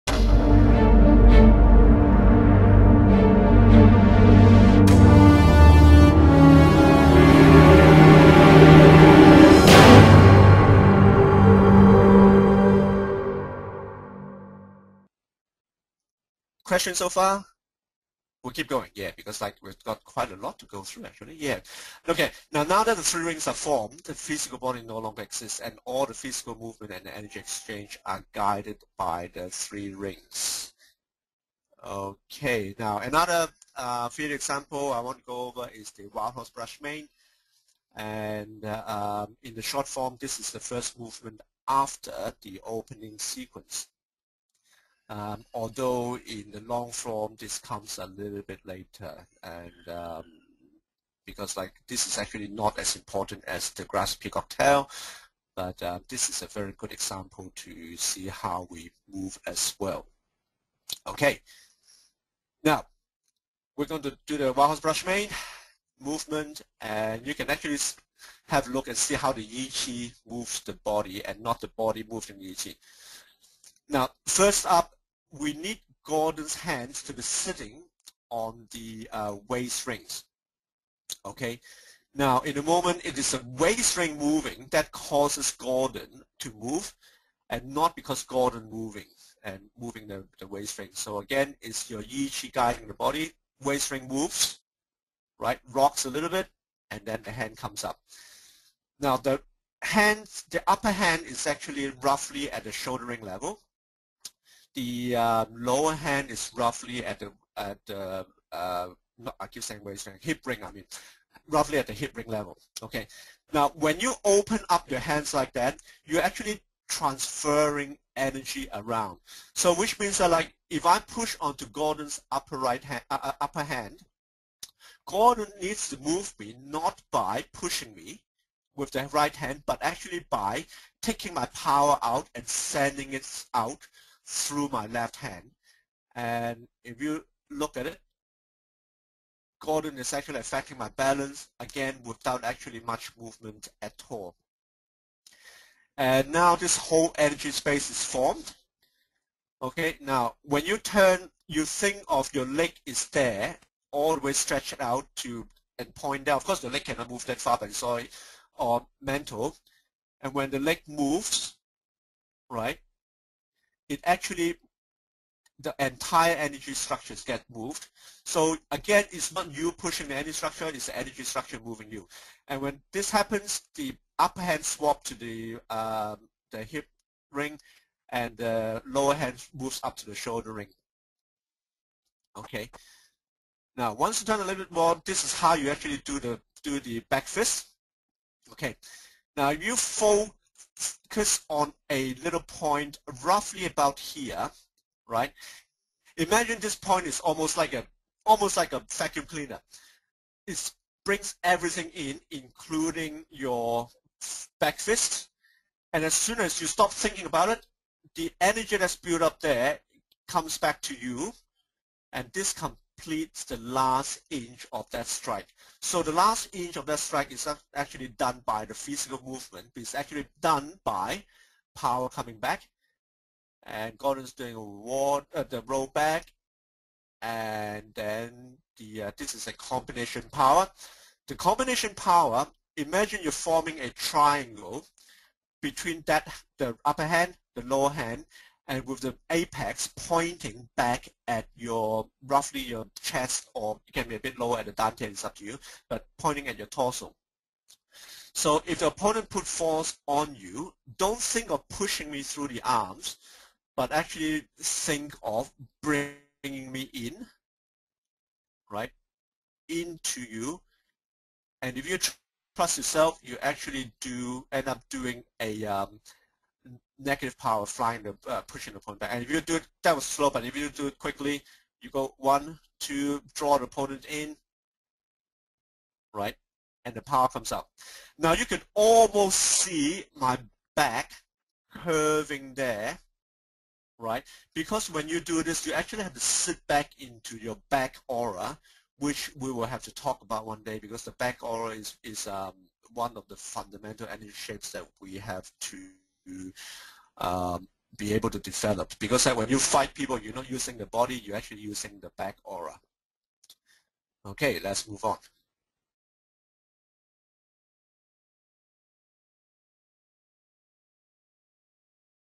John Fung on 3 Rings of Yang Jian Hou Parting Horse's Mane
with presenter John Fung explains the way the 3 rings are employed during Tai Chi's Parting Horse's Mane